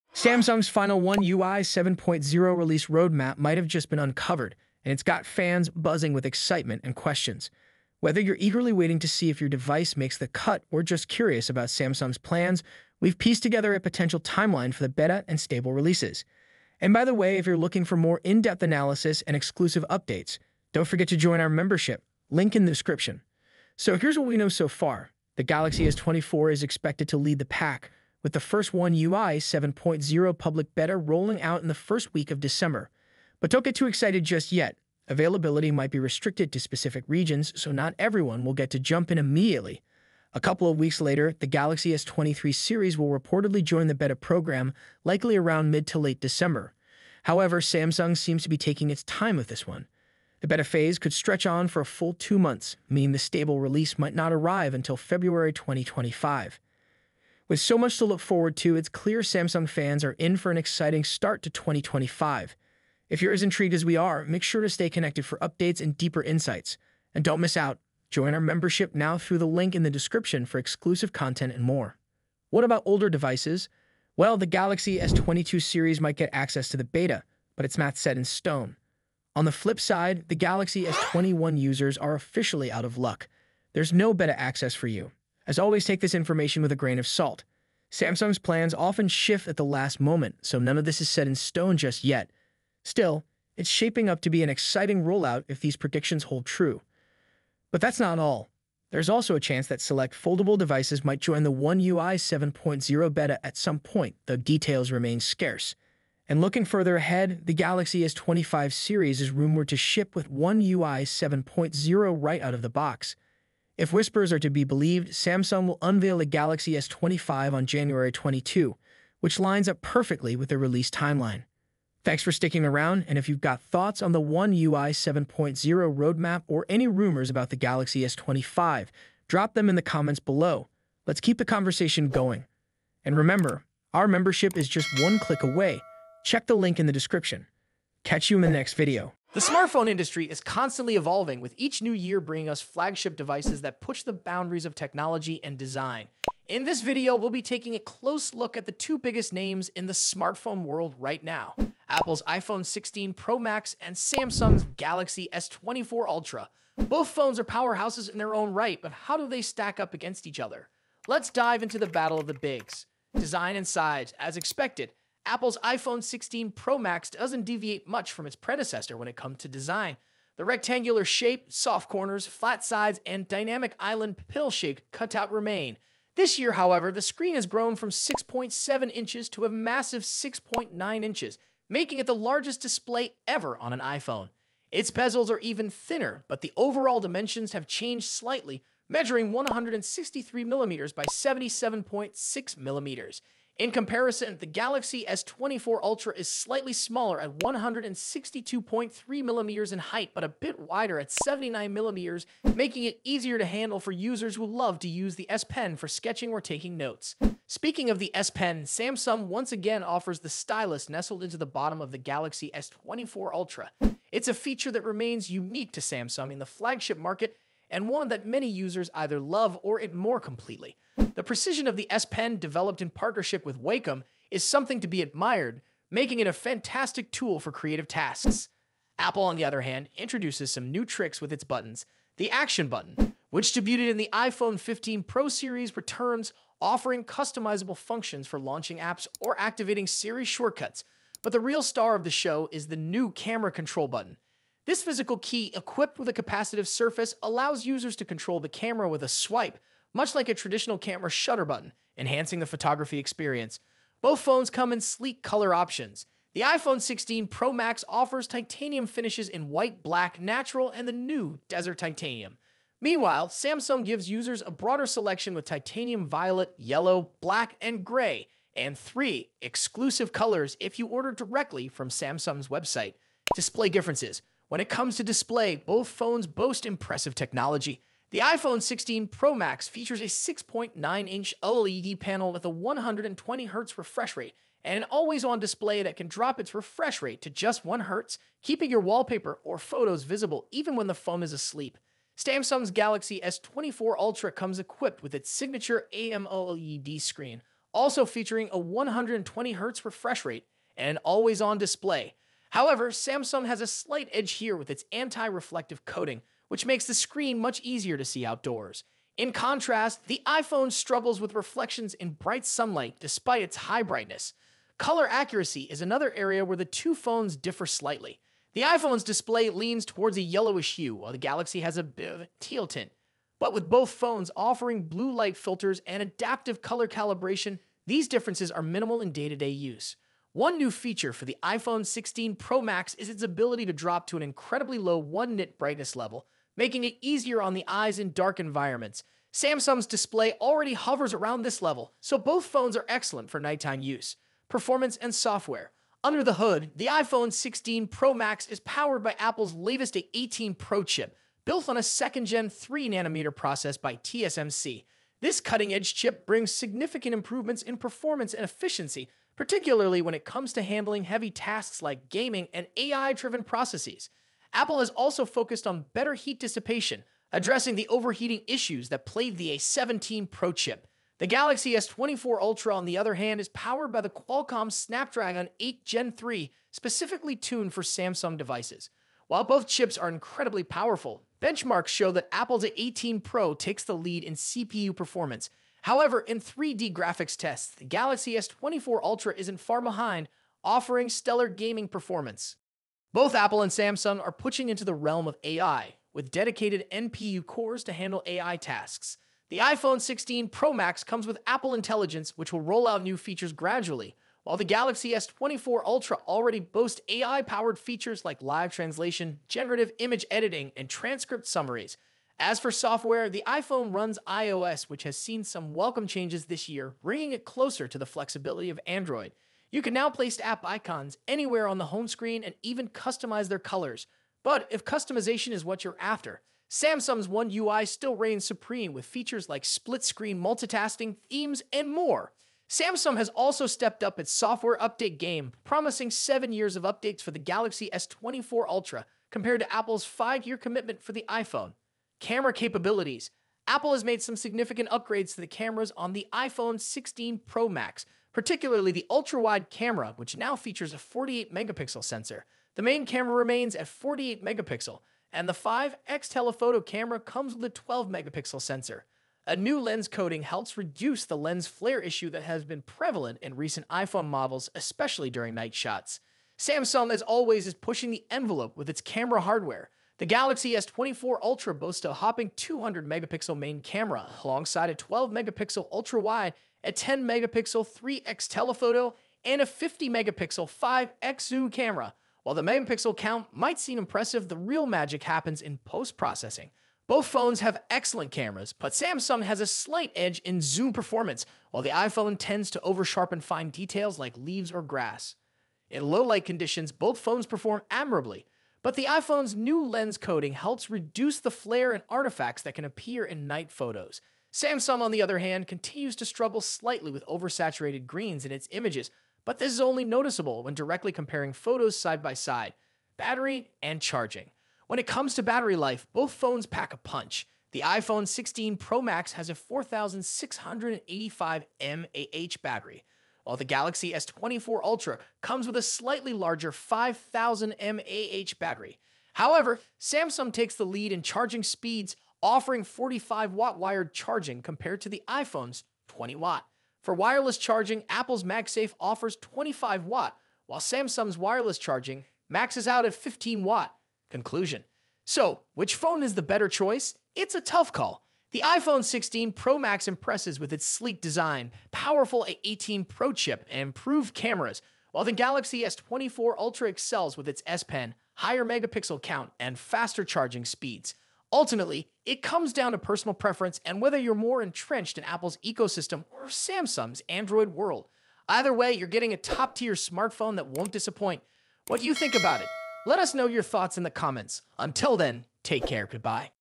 OMG! Samsung One UI 7.0 Beta is FINALLY Dropping! Here's When!
Samsung's secret One UI 7.0 roadmap is finally here! Discover the release dates for the Galaxy S24, S23, S22, and other Samsung devices. Will your phone get the update? Find out now!